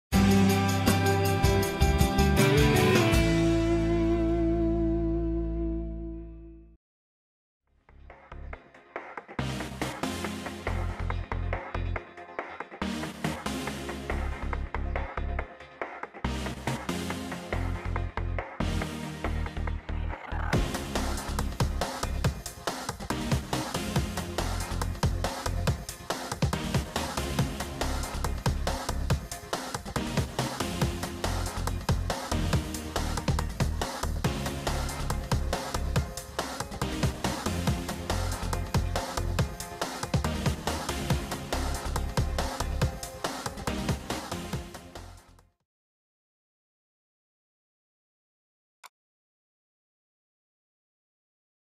80695 Bernoulli-Ball
Der Ball schwebt und tanzt in der Luft. Erklärt wird dieses Phänomen durch den Bernoulli-Effekt. Der Luftstrom hebt den Ball an und umströmt ihn, wodurch der Ball gehalten wird. Wenn Sie langsam den Strom reduzieren, können Sie den Ball auch wieder sicher landen. Auch wenn Sie den Neigungswinkel verändern, bleibt der Ball im Luftstrom.